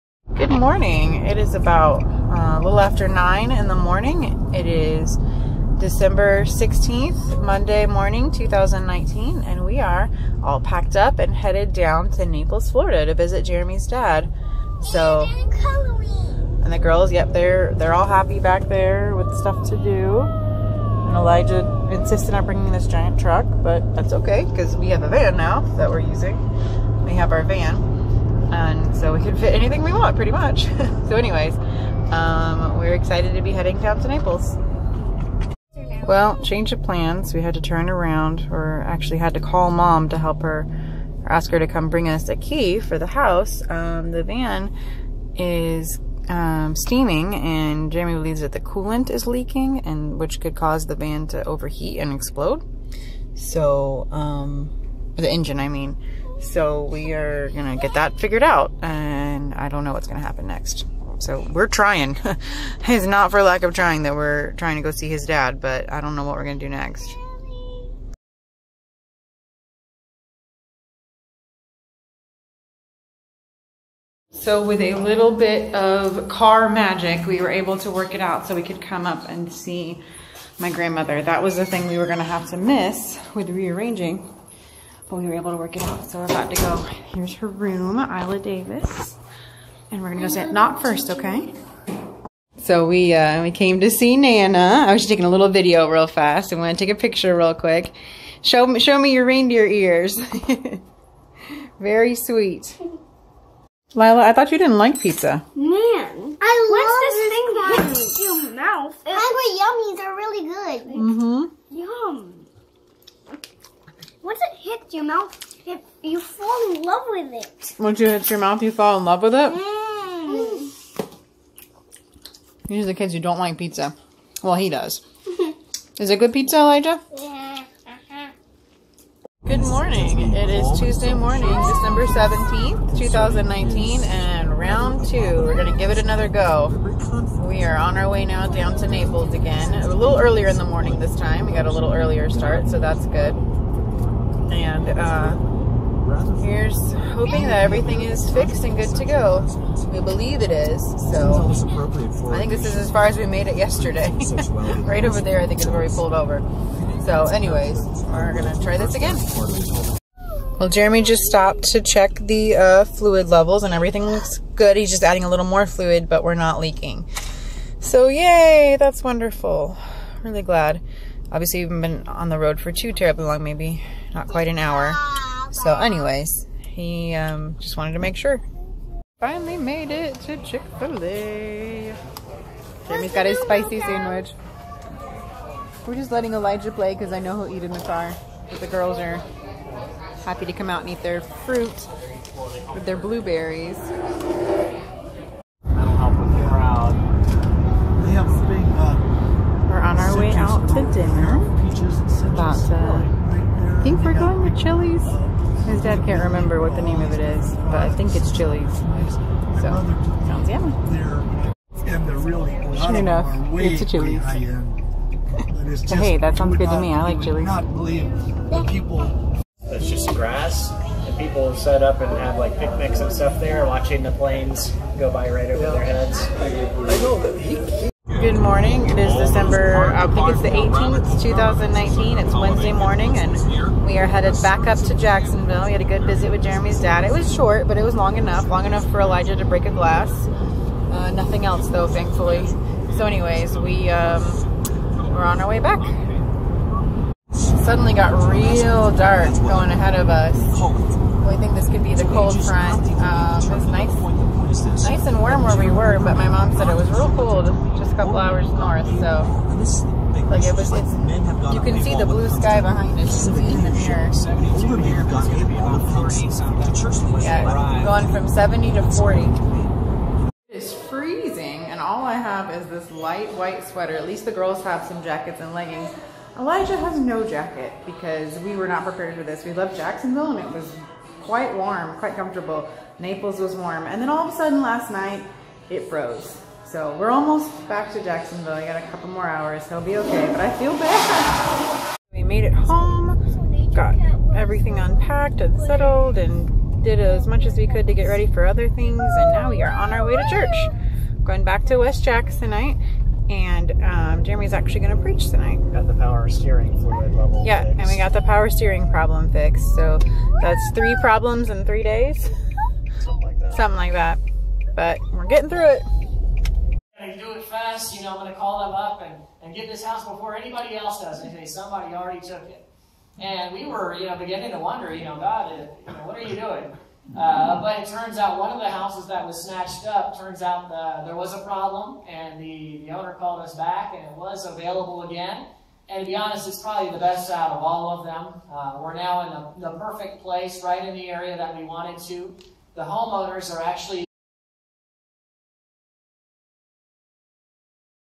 We try to leave for Naples and then we try again (12/16/19-12/18/19)
We pack up to head down to see my husband's Dad but then have to turn around. My husband fixes our van problem and then we head down the next day.